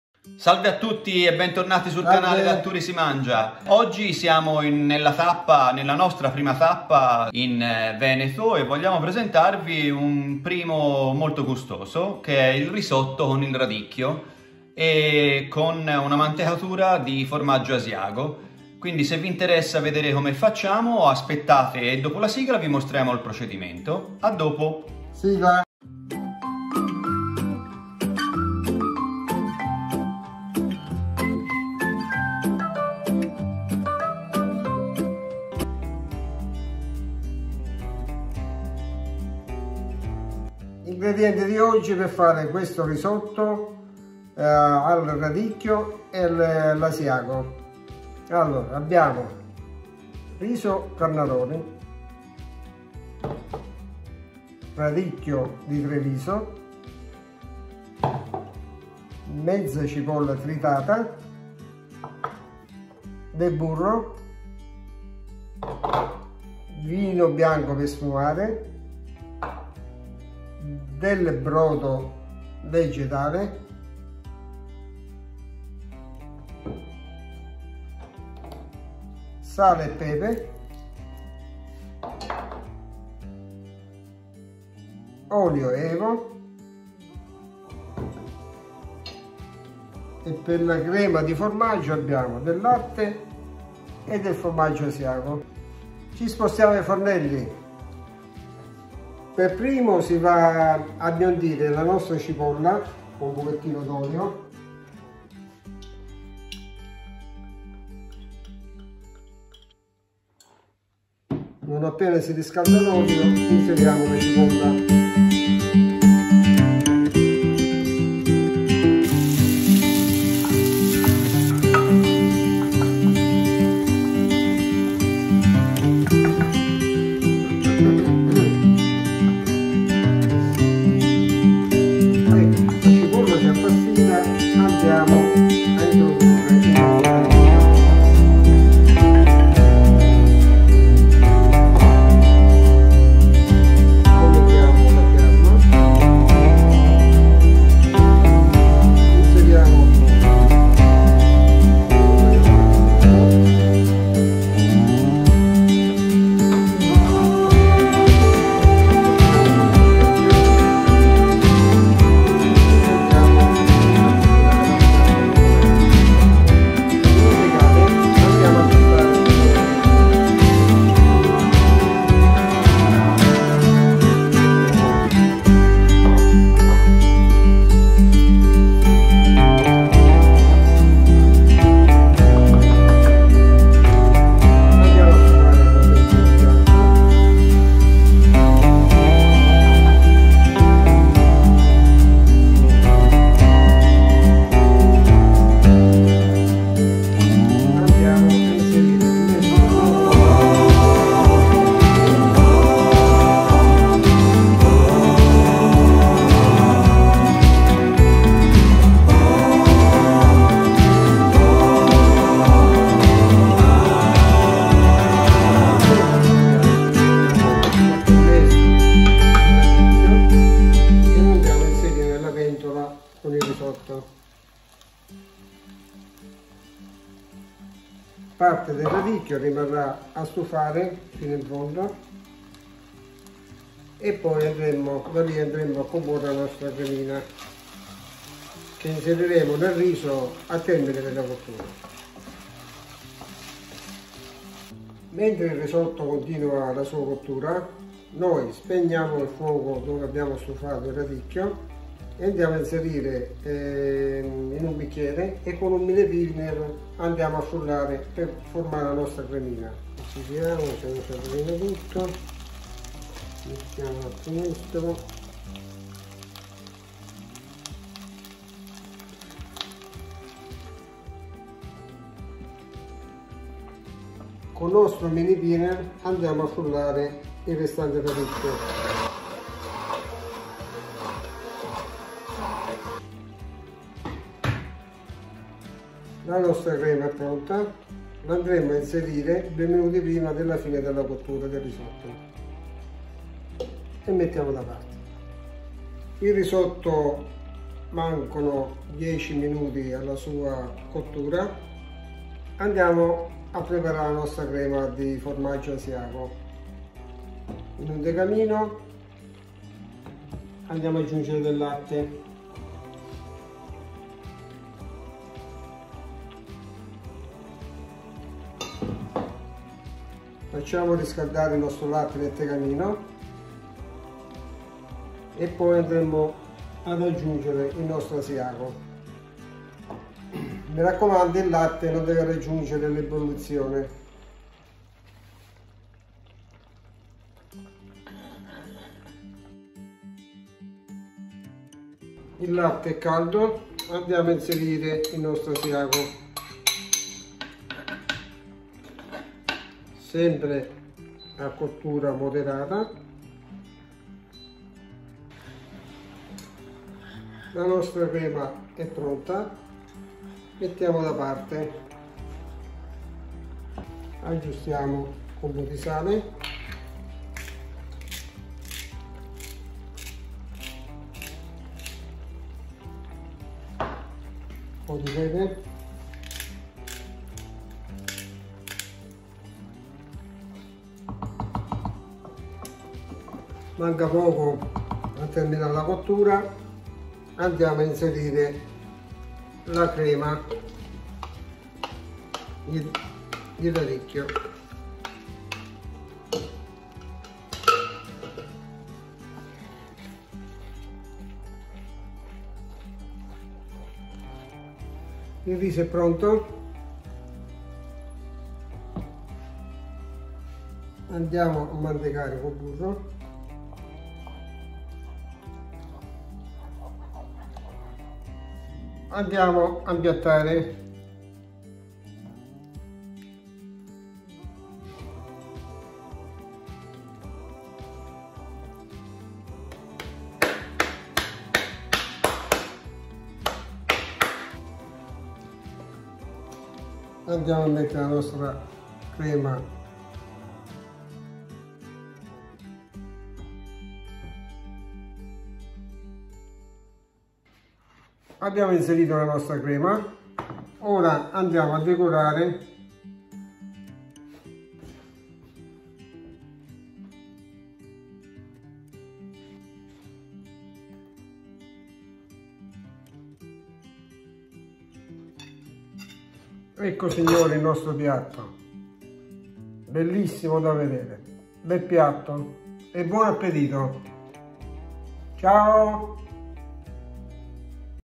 Risotto al radicchio di Treviso mantecato all'Asiago.
Tour culinario italiano. Sesta tappa: Veneto
Ricetta 1/3 - Risotto al radicchio di Treviso mantecato all'Asiago.

Ingredienti.

Per il risotto:

320 gr di riso Carnaroli
250 gr di radicchio rosso di Treviso
1 scalogno o 1/2 cipolla media
30 gr di burro
1/2 bicchiere di vino bianco secco
1,5 lt di brodo vegetale

Per la crema di taleggio:

150 gr di formaggio Asiago
75 ml di latte intero fresco

Seguiteci anche sulla nostra pagina               Instagram: da_tury_si_mangia
                                                                            su facebook: Da Tury Si Mangia